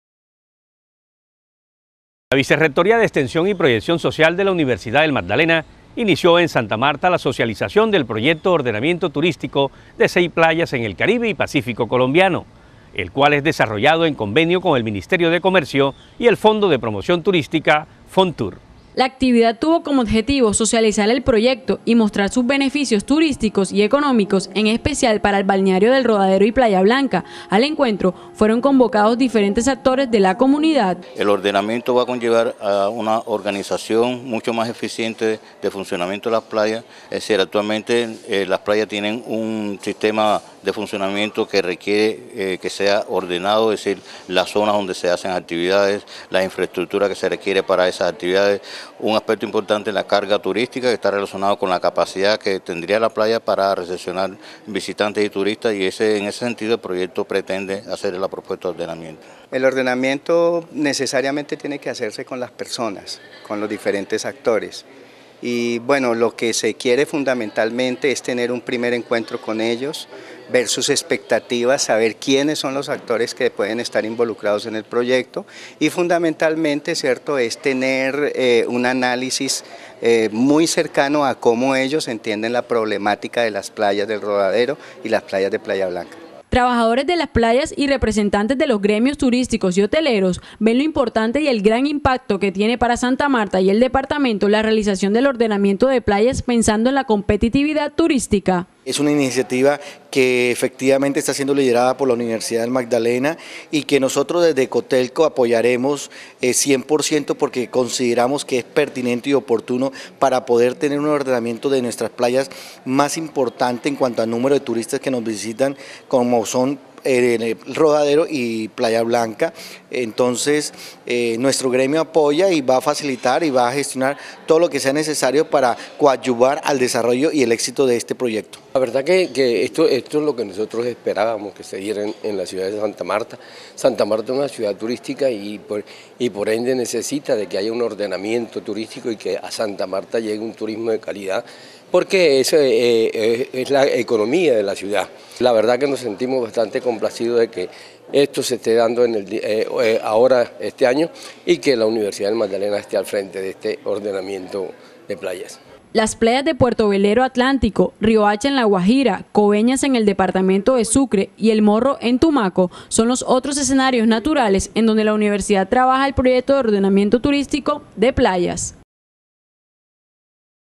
Socialización del proyecto de ordenamiento turístico de 6 playas en el caribe y pacifico colombiano
La vicerrectoría de extensión y proyección social de la Universidad del Magdalena inició en Santa Marta la socialización del proyecto de ordenamiento turístico de 6 playas en el caribe y pacífico colombiano.